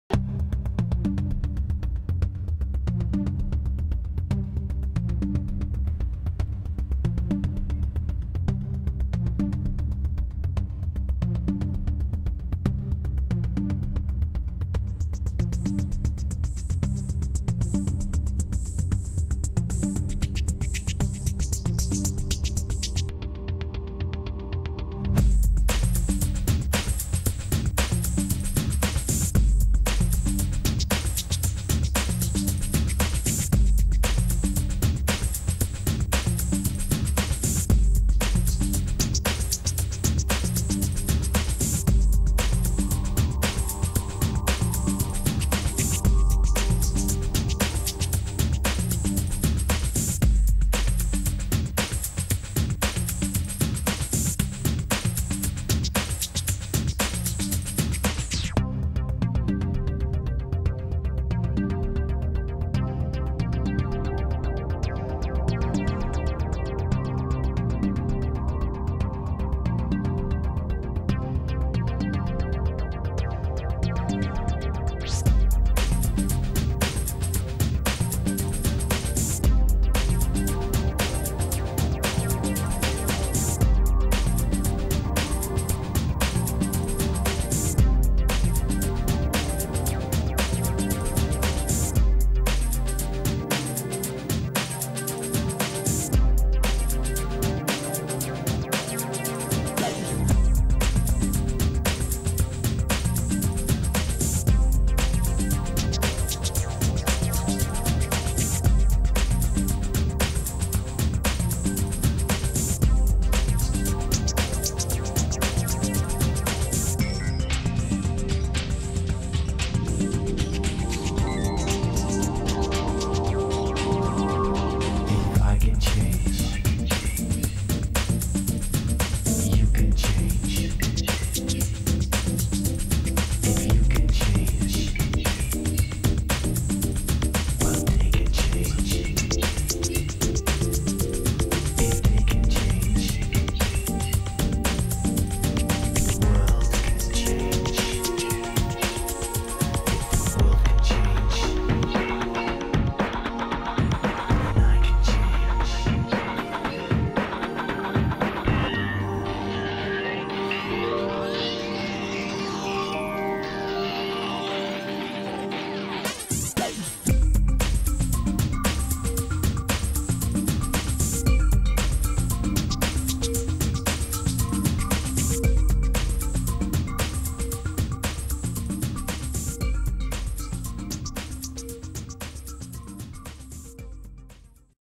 Amazing Tortoise Beetles of Boracay Island Philippines.
Three species of Boracay's Tortoise Beetles and the first full documentation of their Life Cycle.